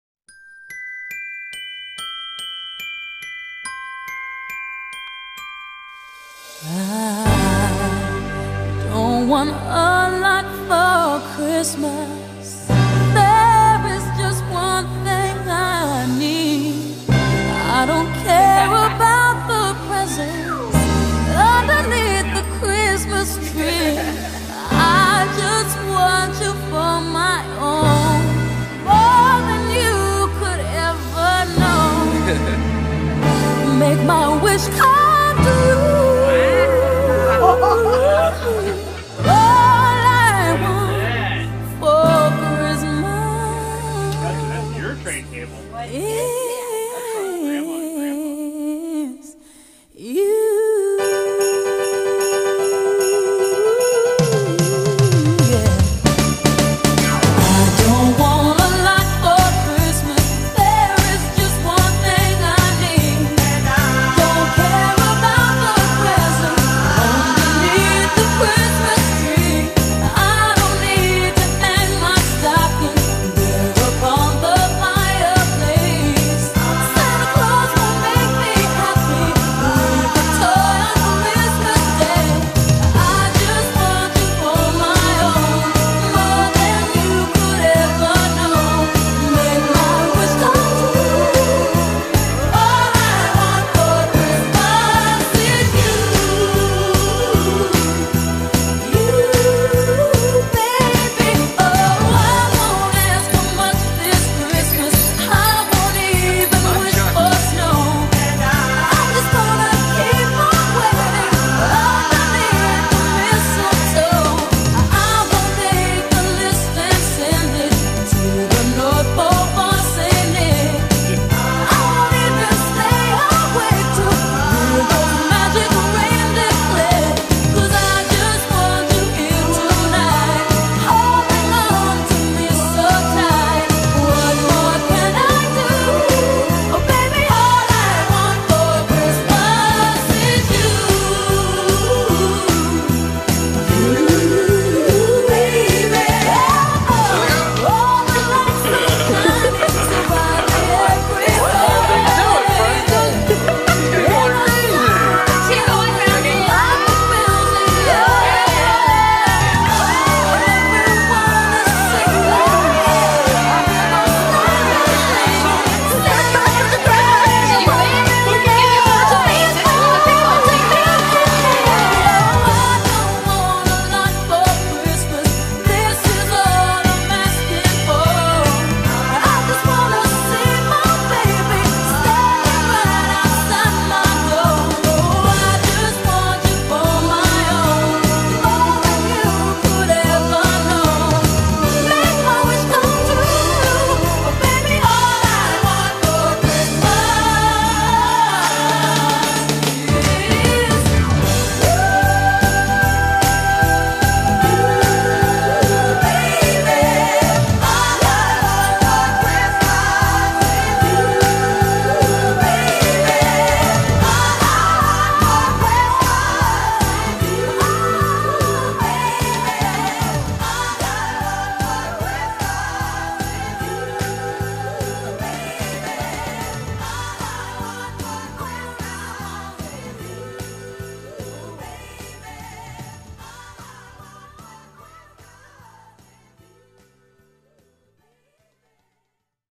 Borchardt Christmas 2012
Mischief abounds at the first Christmas for the newly enhanced Borchardt family!